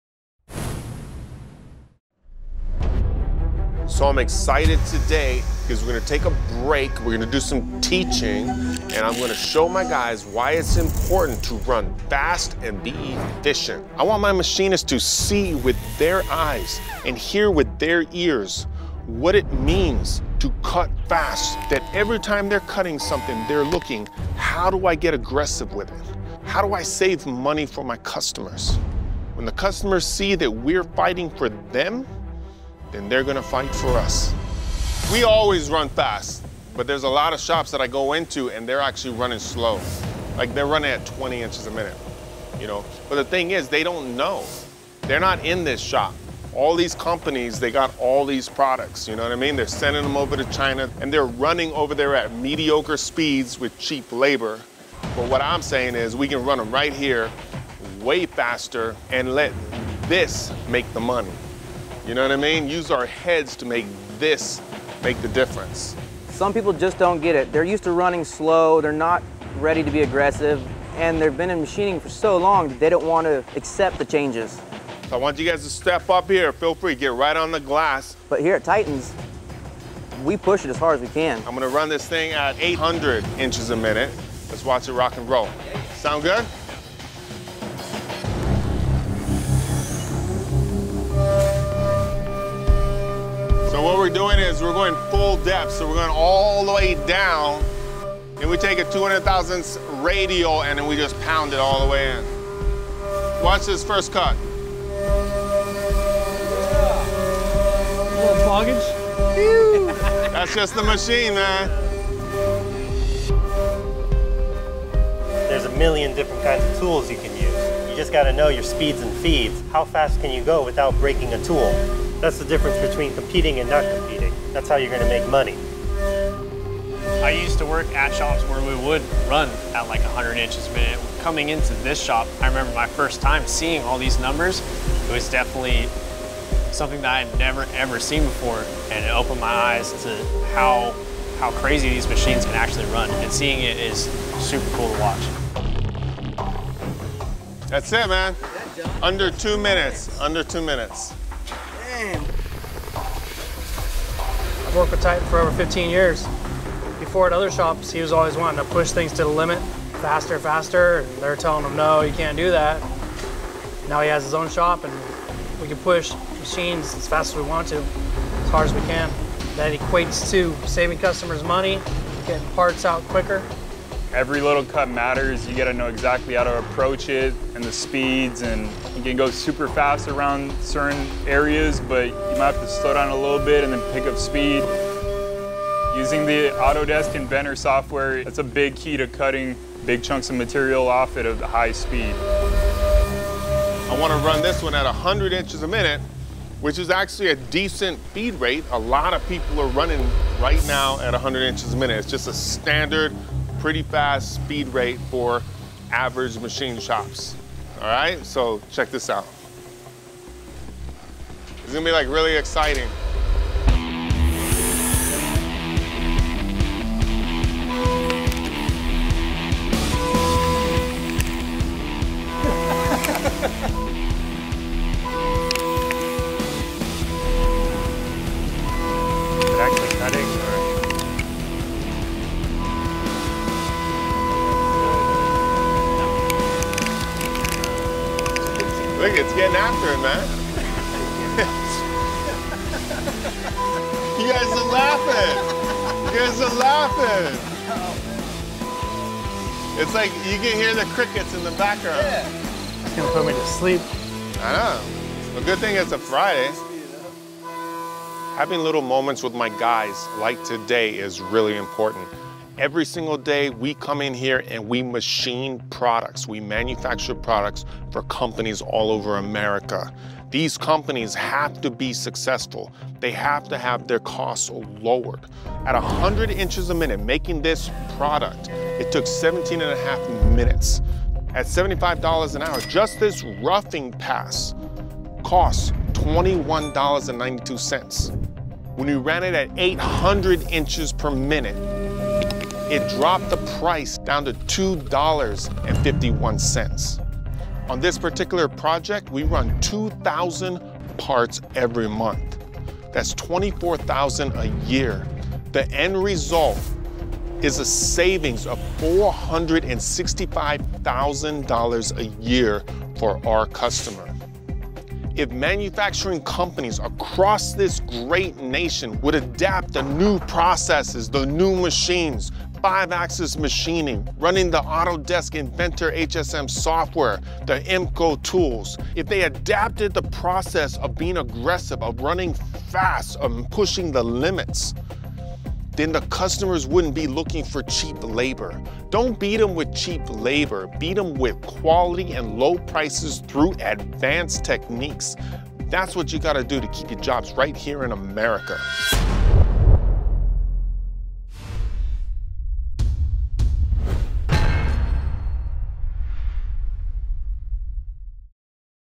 Pushing CNC Machine Speeds to the Limit
TITAN Gilroy talks to his guys about how to run the cnc machines faster, which means saving customers money and getting parts out quicker.